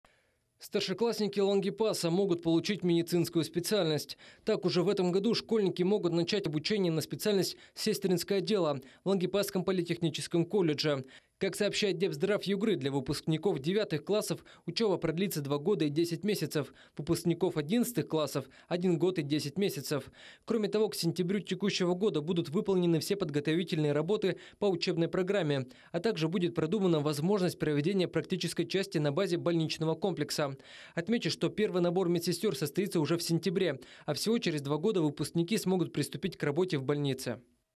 В Лангепасском политехническом колледже будут обучать медсестёр
Старшеклассники Лангепаса смогут получить медицинскую специальность.